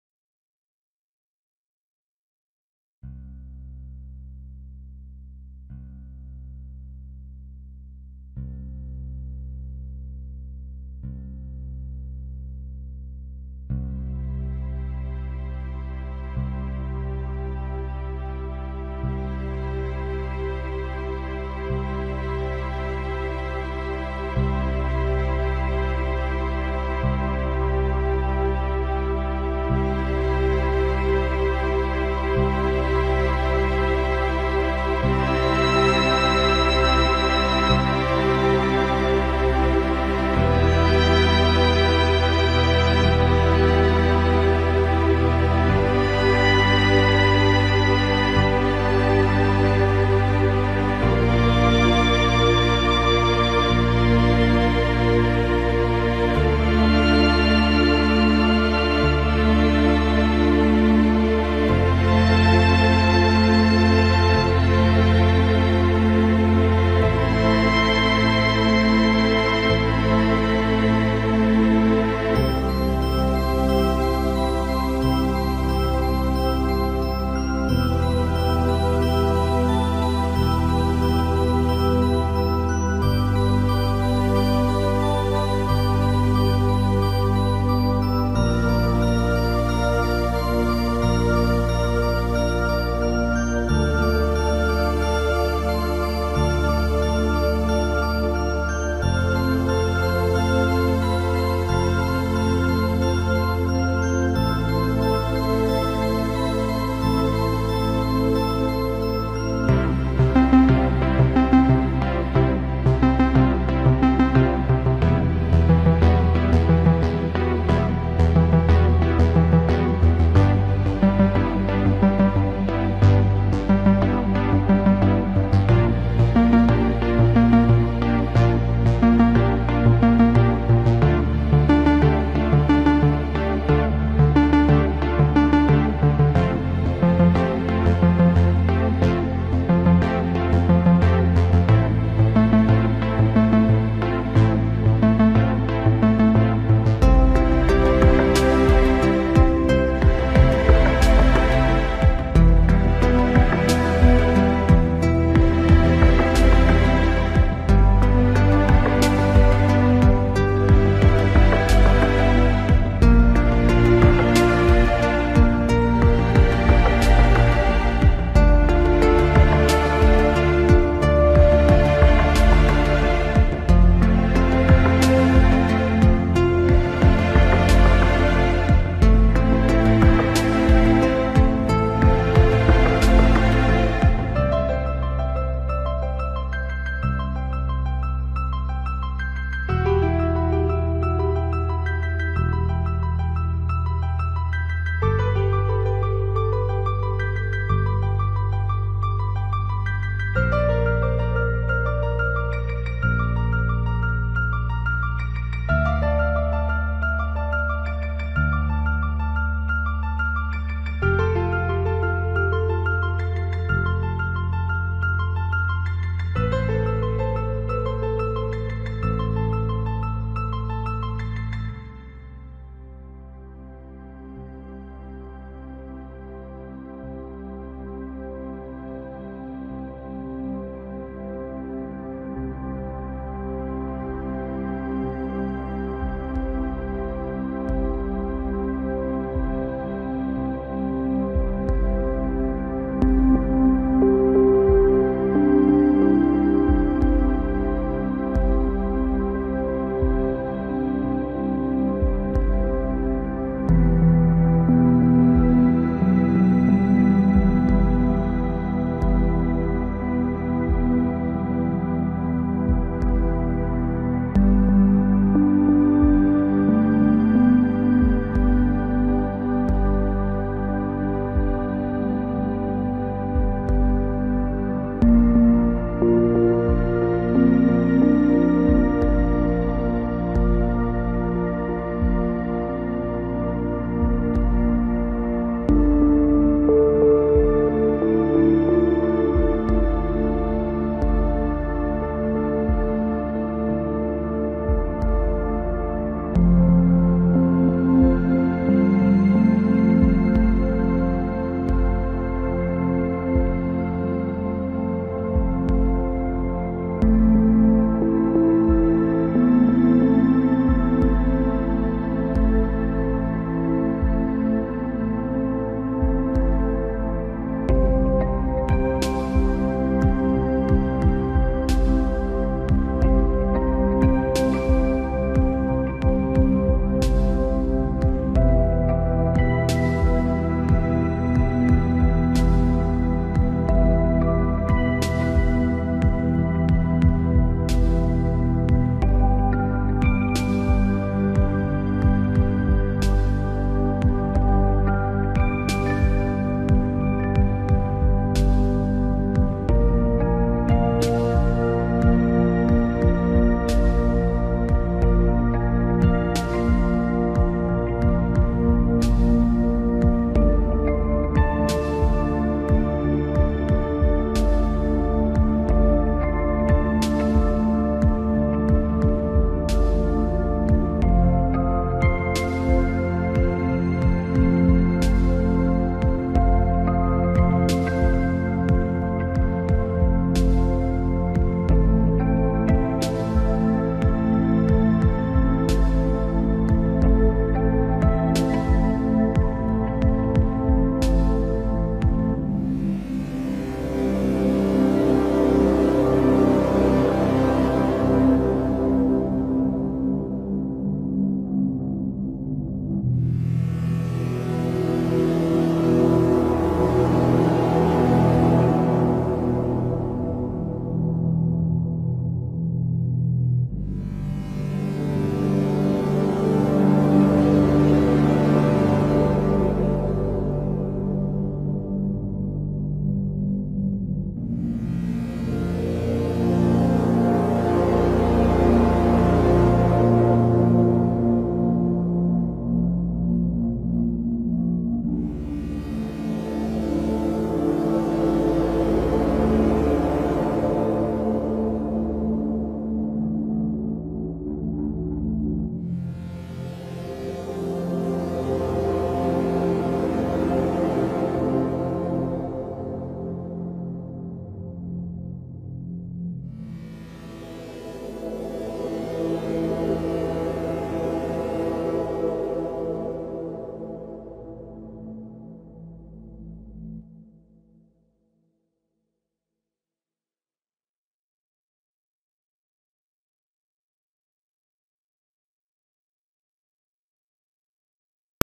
Der Himmel über Seeheim | Zeitraffer | Time Lapse
Mit Zeitintervall von fünf Sekunden ein Bild geschossen und mit 24fps wiedergegeben.

Kamera: Fujifilm FinePix HS20EXR in Kombination mit dem
JJC TM-K Timer (Als Ersatz für den Fujifilm RR-80)
Geschnitten mit Lightworks Pro.